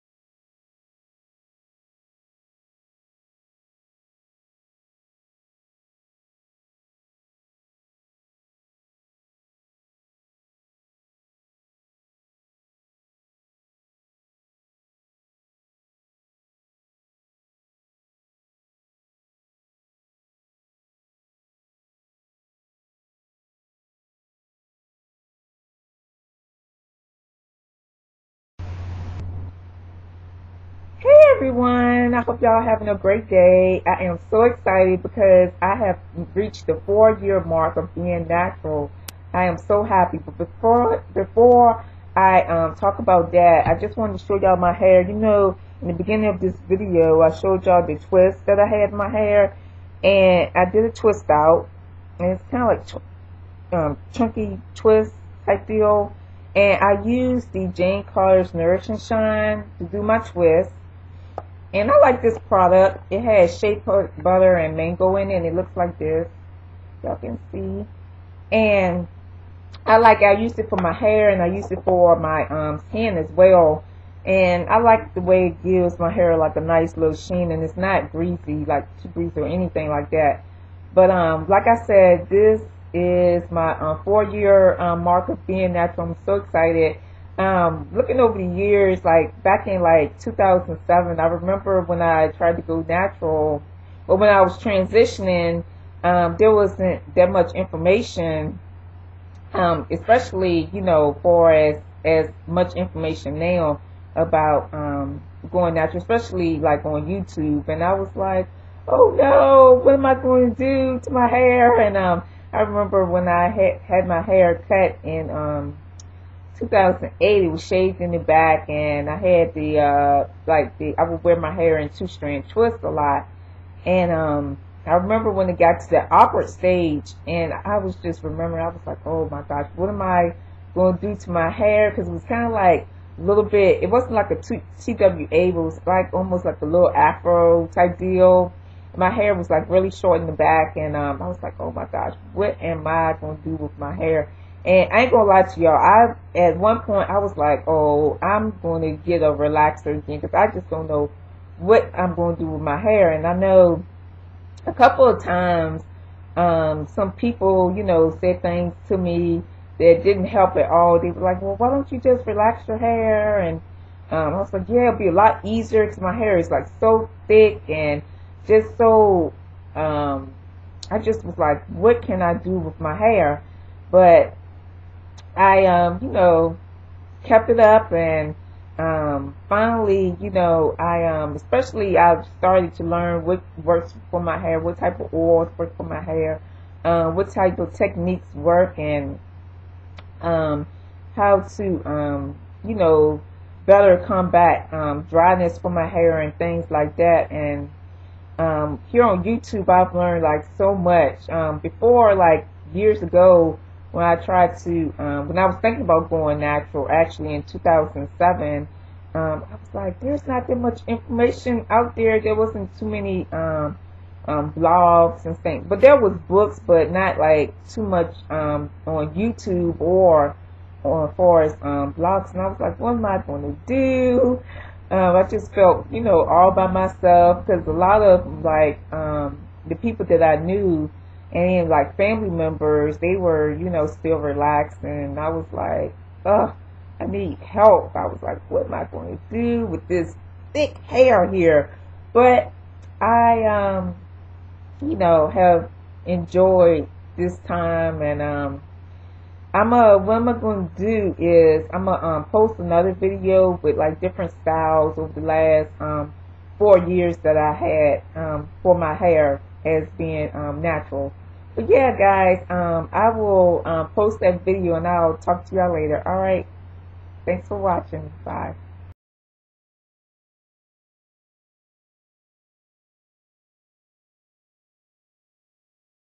Four years natural and chunky twists
In this video, I talk about my transition to natural hair in 2007. It has been over four years since I had my last relaxer. I discuss my 4 year natural hair journey. I talk about some of the challenges of going natural and at times not knowing what to do with my thick hair. I even talk about going back to relaxed hair. My hair is in chunky twist and I used Jane Carter Solutions Nourish and Shine to twist my hair. For those that are wondering if I had to classify my hair I would say that it is type 4 with a mixture of 4a, 4b, and 4c.
I classify my "natural hair as 4b."